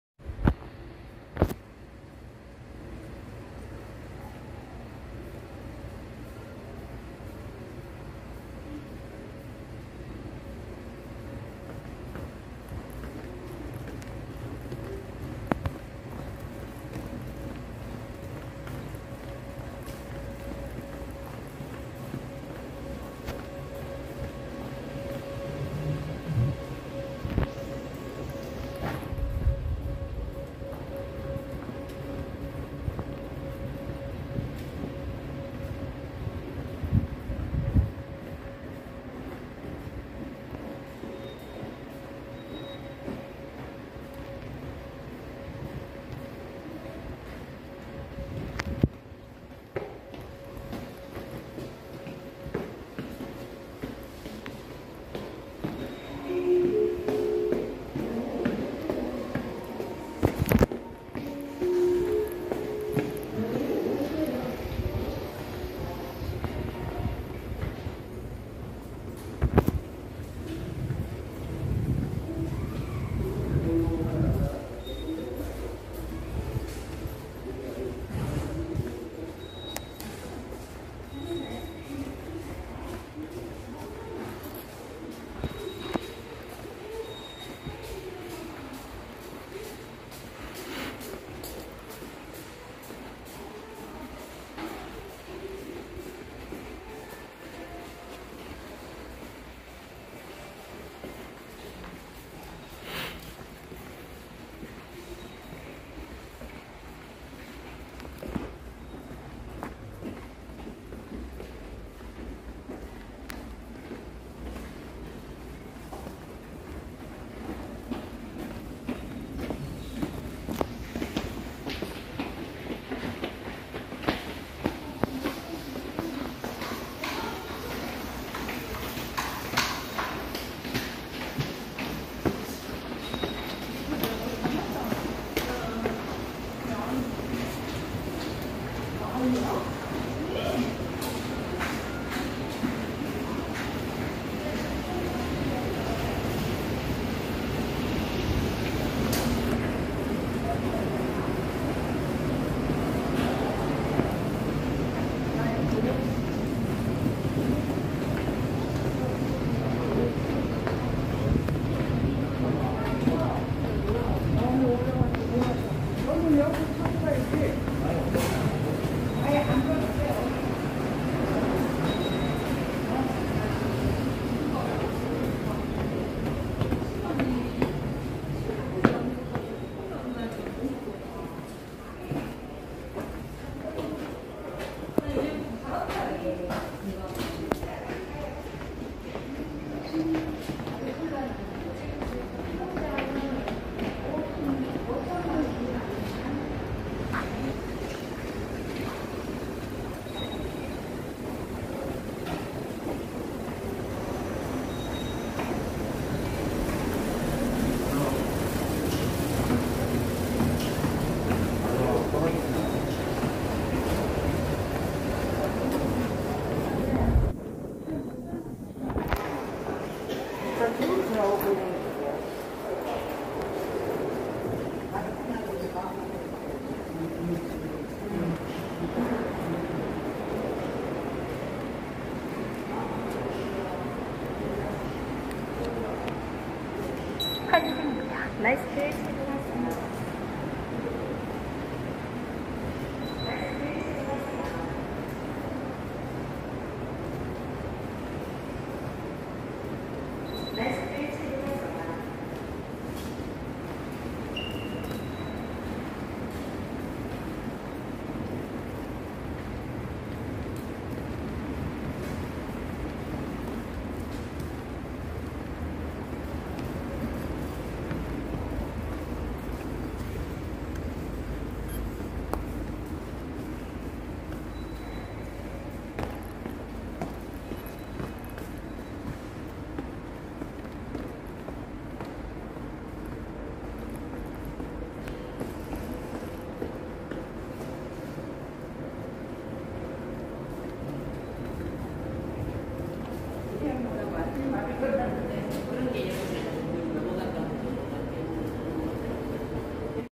[환승] 서울 도시철도 신설동역 2호선→우이신설선
1호선 위로 지나가는 구조라 시간이 살짝 걸리지만 계단은 많이 없어서 무난했습니다

해당 영상을 2차 창작에 사용할 때에는 출처를 표기해주시고, 악의적 목적으로 사용하지 말아주시기 바랍니다.